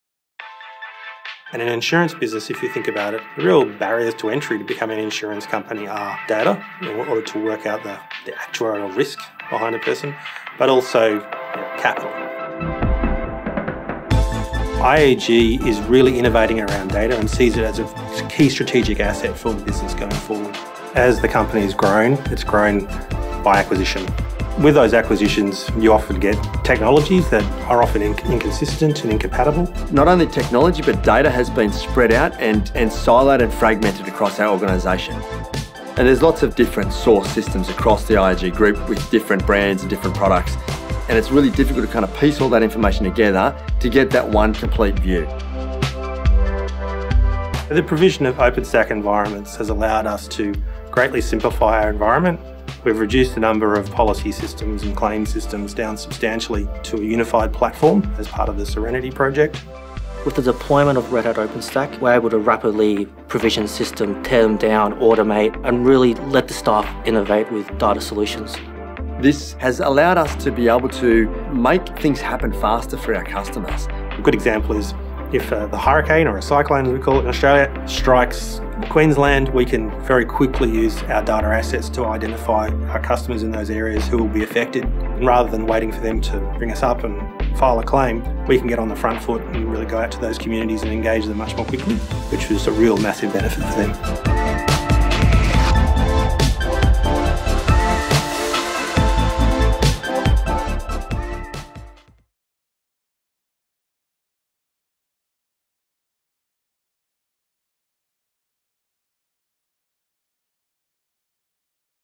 IAG unifies its IT platform, innovates with data solutions
To simplify its data infrastructure and deliver better customer experiences, IAG streamlined its technology and became a more data-driven company.